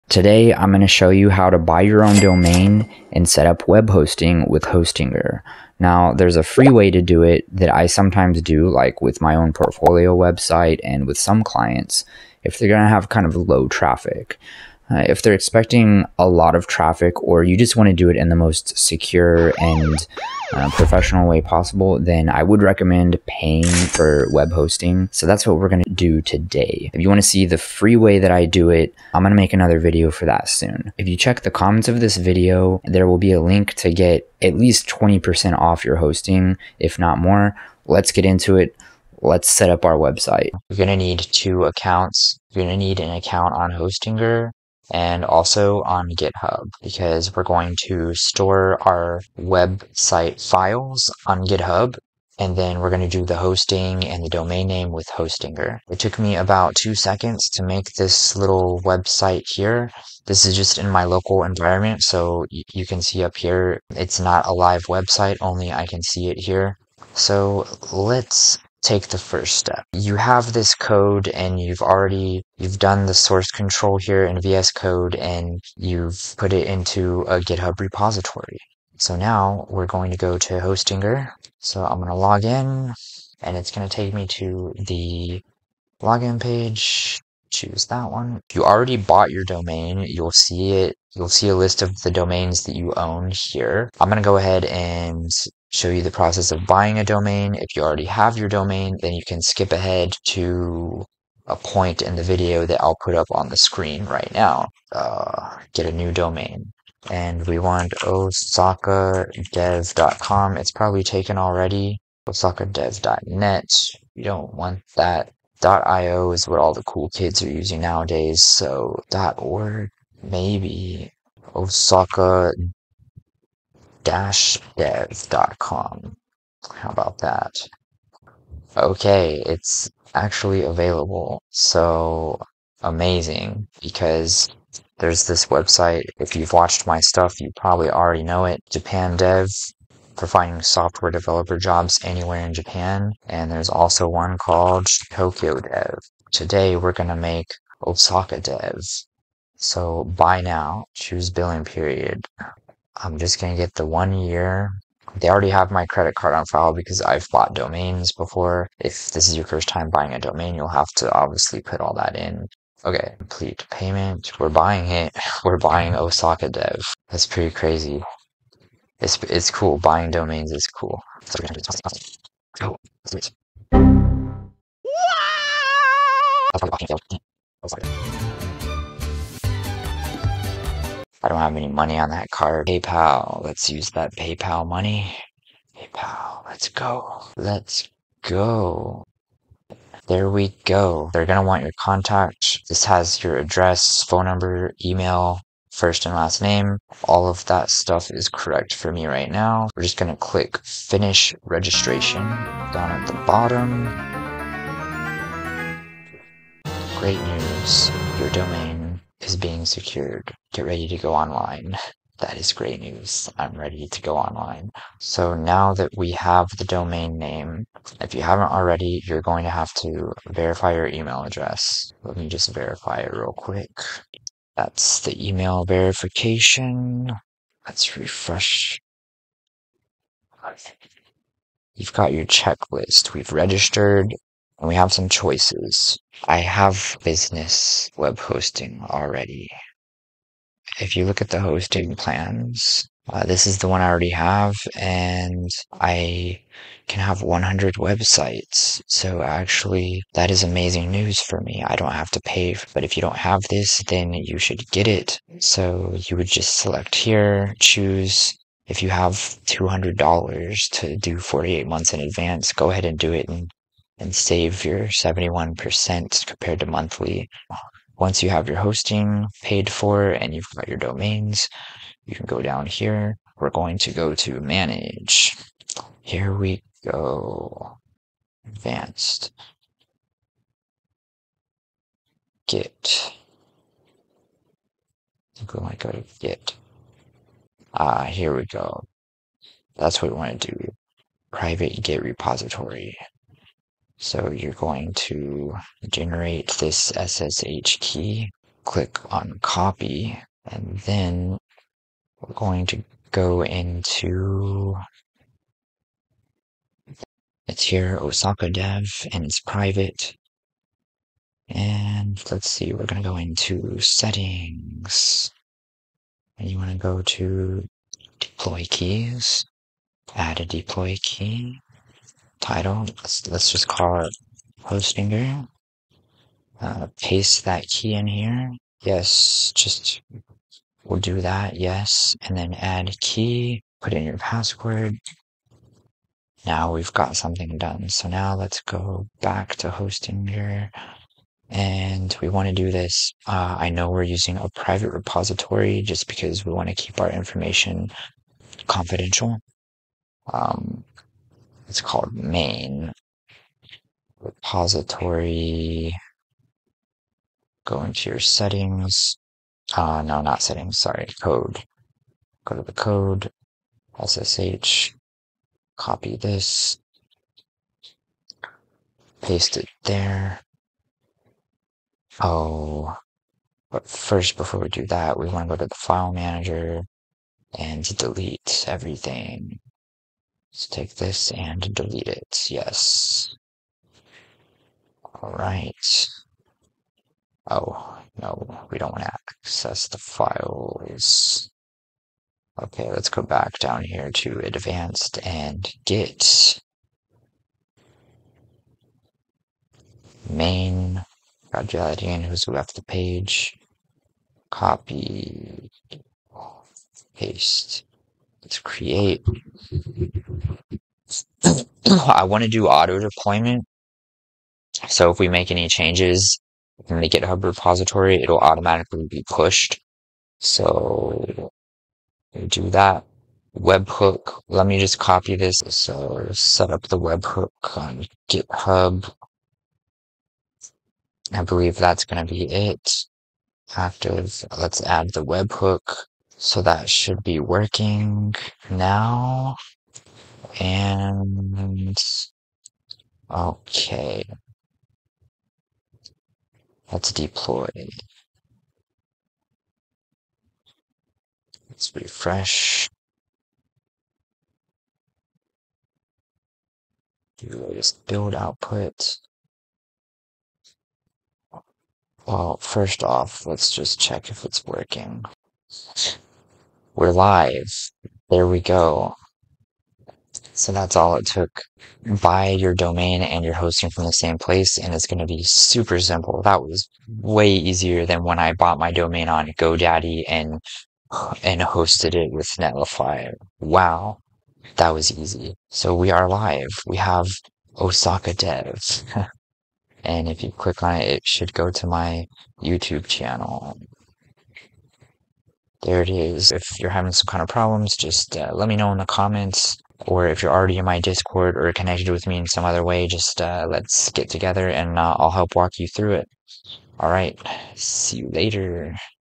How to host a website with Hostinger
Website hosting with Hostinger:
Full-time Software Engineer shows you how to do web hosting with Hostinger.

Chapters:
0:00 Free vs. paid web hosting
0:40 GitHub and Hostinger
1:45 Buying a domain
3:51 Buy Hostinger web hosting
4:38 Generate SSH key
5:10 Add deploy key
5:40 Create Hostinger repository
6:52 set up auto deployment
7:25 Deploy repository
7:45 Your website is now on the web!

Your website will be up and running in 15 minutes with this tutorial!

Setting up your website with Hostinger is easy if you buy both your website domain and website hosting from Hostinger.

If you've never set up a before, do not worry.